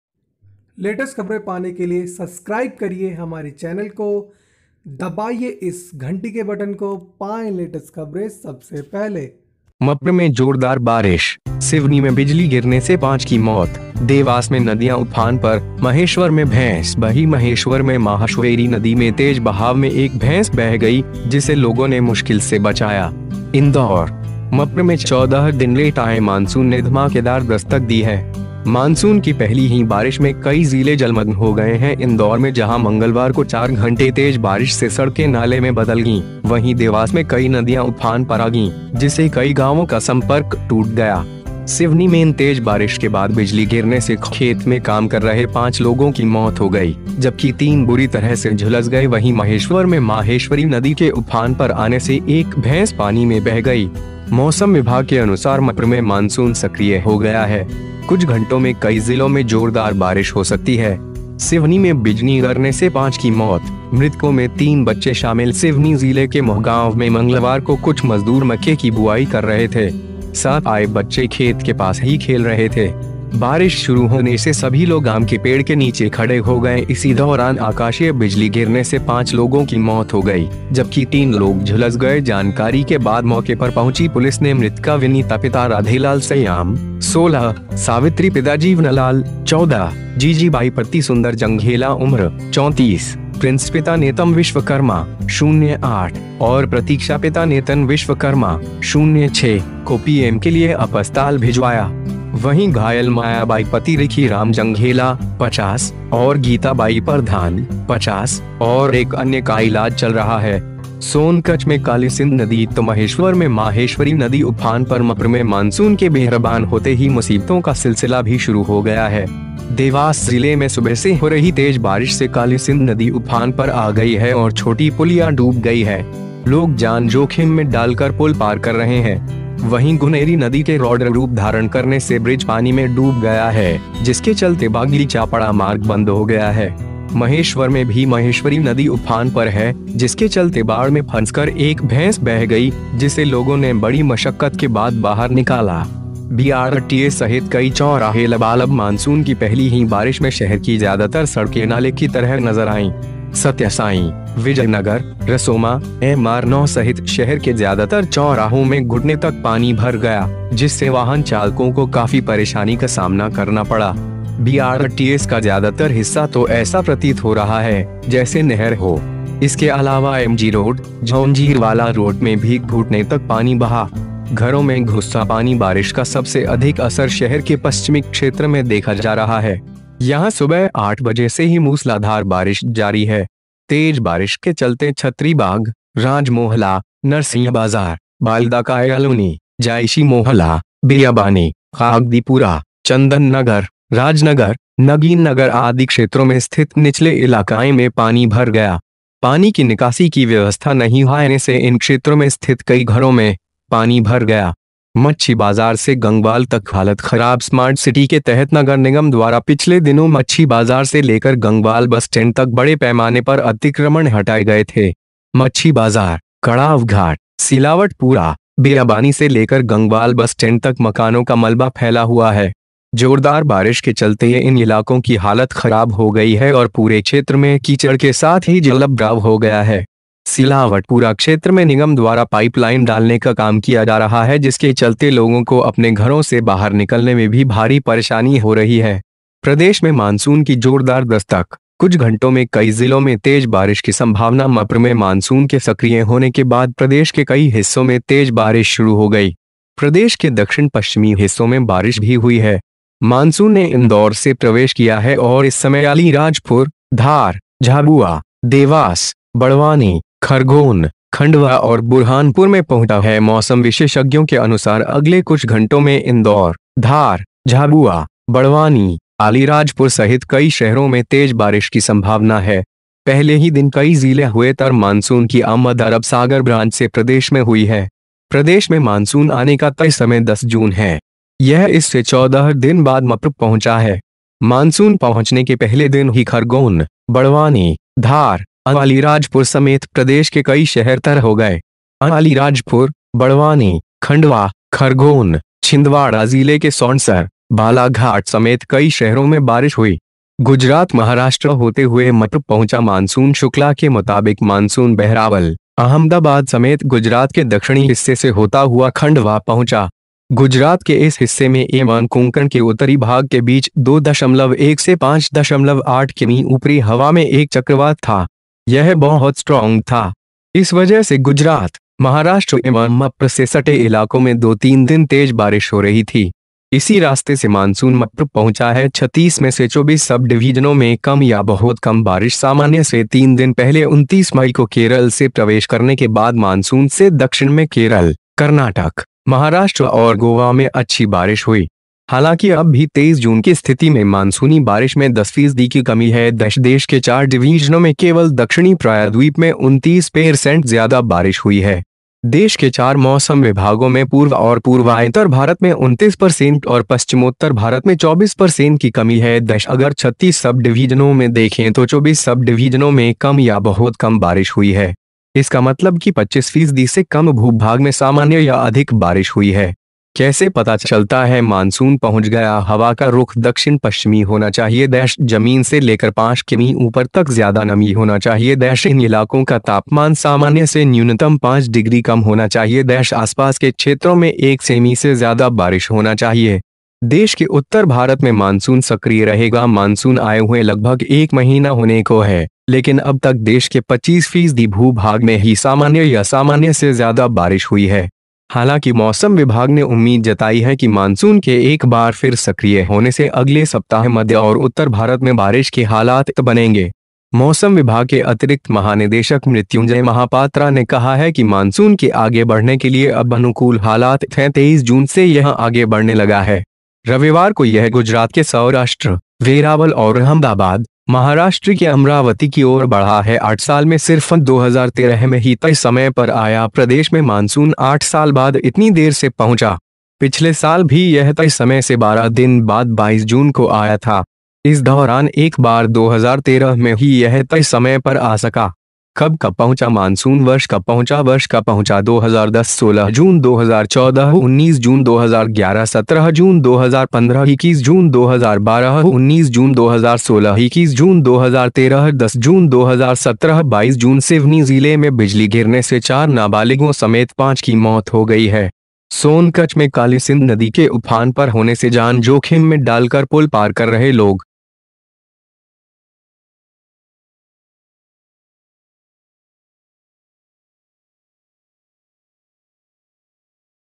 मप्र में जोरदार बारिश, सिवनी में बिजली गिरने से 5 की मौत, देवास में नदियां उफान पर, महेश्वर में भैंस
मप्र में जोरदार बारिश, सिवनी में बिजली गिरने से 5 की मौत, देवास में नदियां उफान पर, महेश्वर में भैंस बही